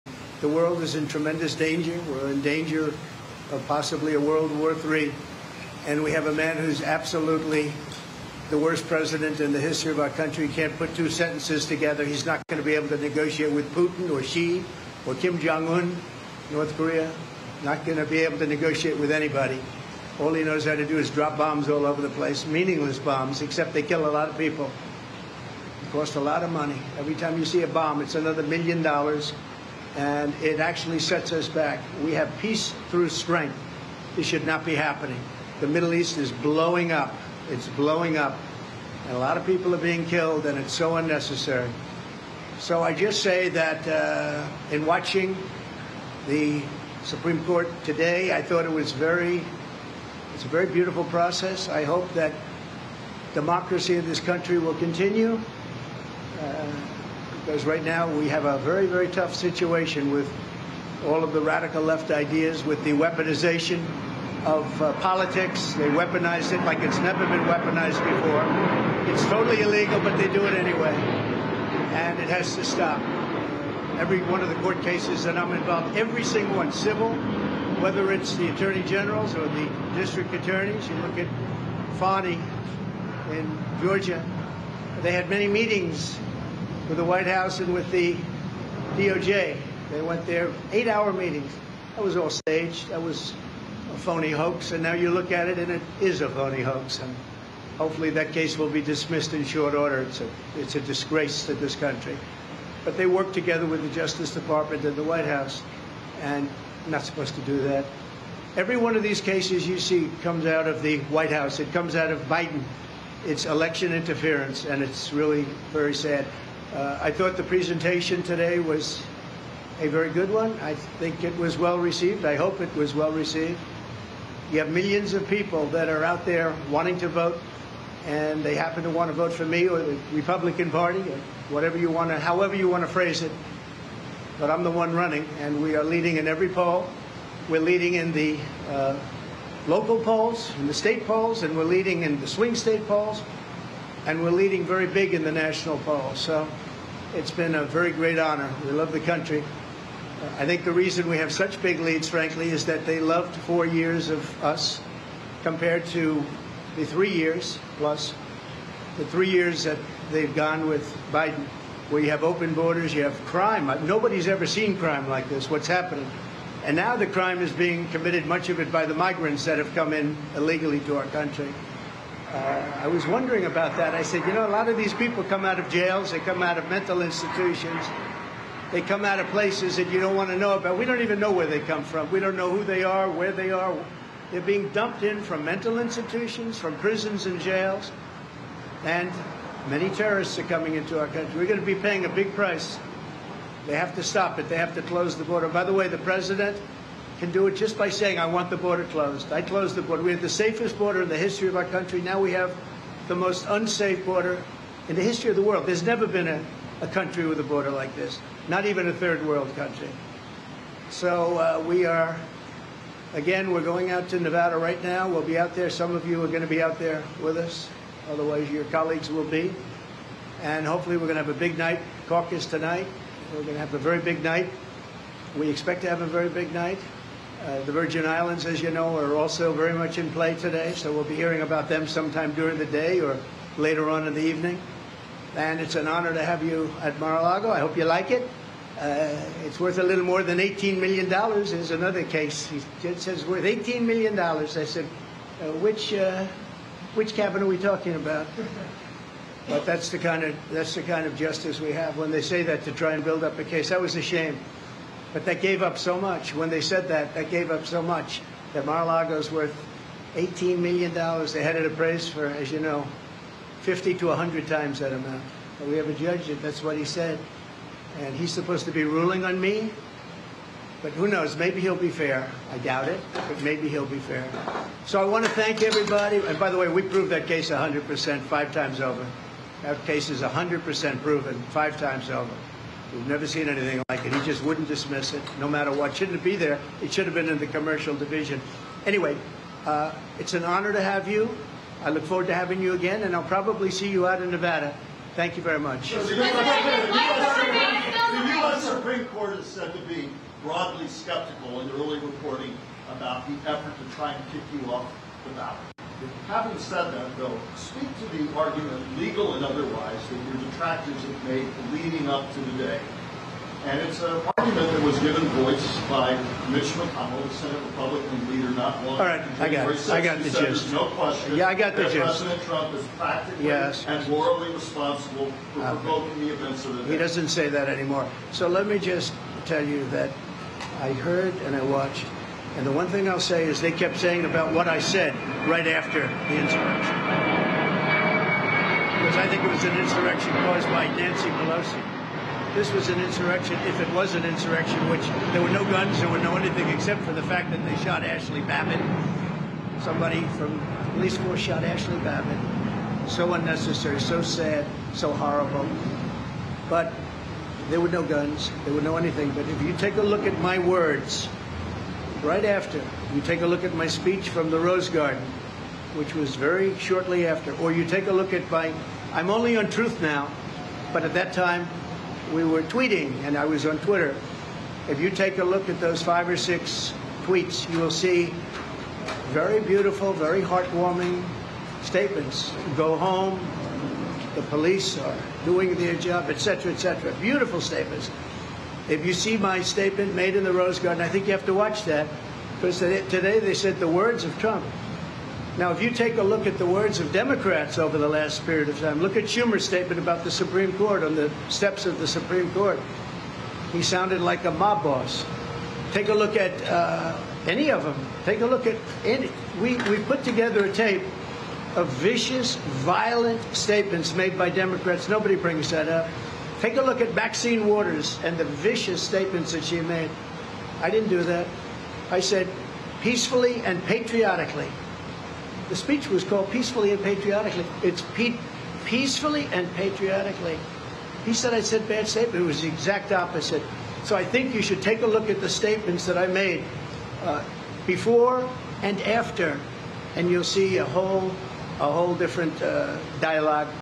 Trump: Biden 'can't put two sentences together' as world could face 'WWIII'
Donald Trump has accused Joe Biden of not being able to string a sentence together and making the world a more dangerous place.

The former President was speaking as the US Supreme Court rules on a decision made in Colorado which prevents Mr Trump from appearing on the state's ballot for this year's presidential election.